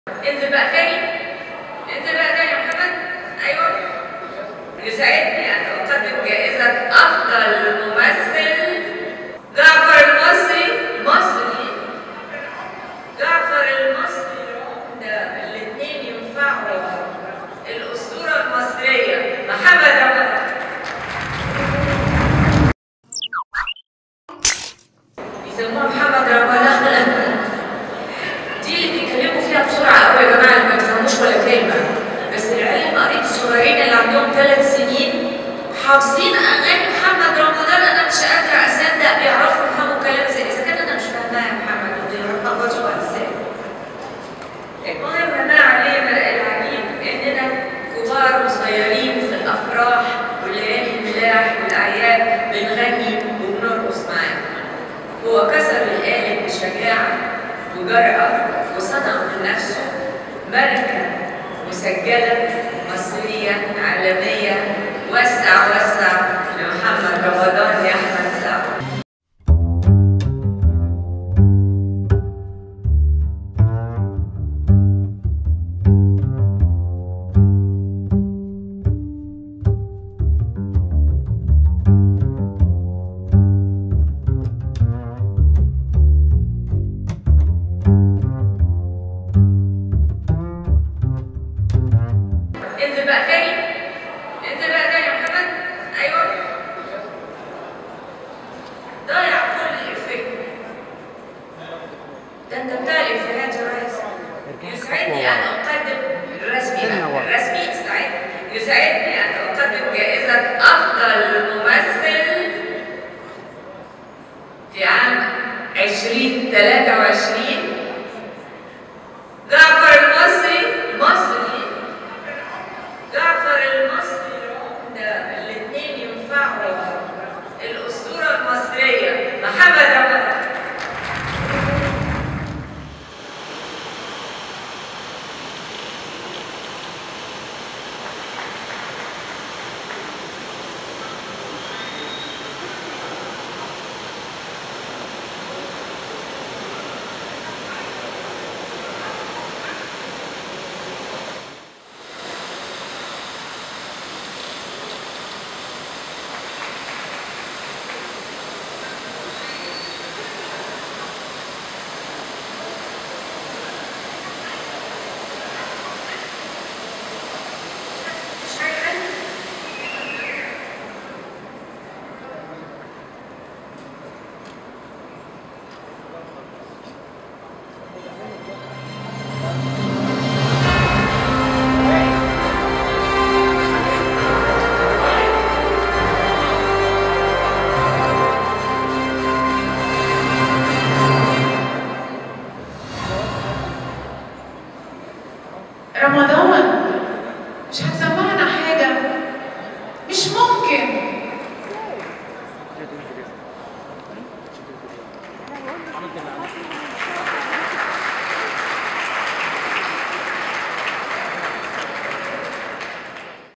علي طريقة هاني رمزي.. هالة سرحان تخطئ في أسم محمد رمضان وتضعه في ورطه بهذا الطلب!
ONE TAKE#الحصري_عندنا